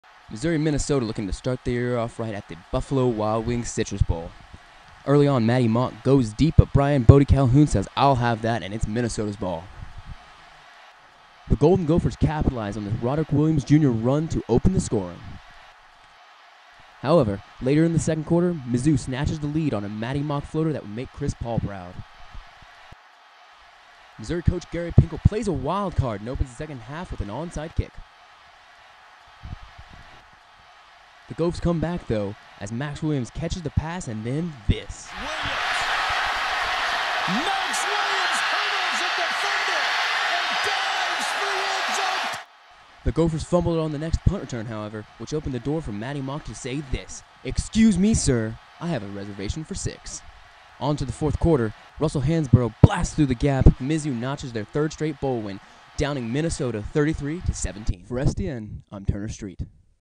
1st Place STN 2015 Sports Highlight
The spring 2015 Student Television Network sports highlight competition winning video.  Submitted by Turner Street and Eric Goodman of Allen D. Nease High School.  Contestants were given the complete file of a broadcasted sporting event and had to create a 1-minute long highlight with cometary in three hours.